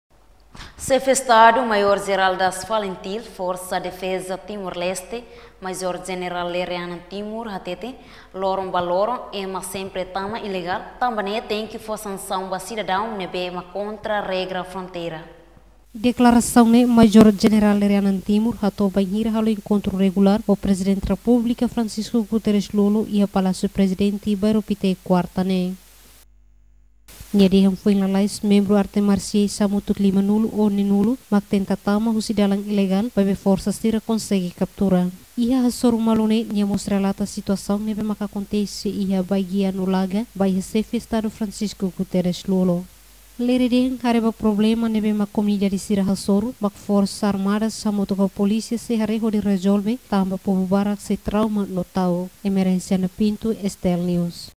Sidadun Tama Ilegal Tenki Fo Sansaun (News 09-09-2020)
Xefi Estadu MaiorJeneral Das Falintil Forsa Defeza Timor Leste (F-FDTL), Maior Jeneral Lere Anan Timur Hatete Loron Ba Loron, Ema Sempre Tama Ilegal. Tanba Nee Tenke Fo Sansaun Ba Sidadaun Neebe Mak Kontra Regra Fronteira.

STL News Kalan, 09 Setembru  2020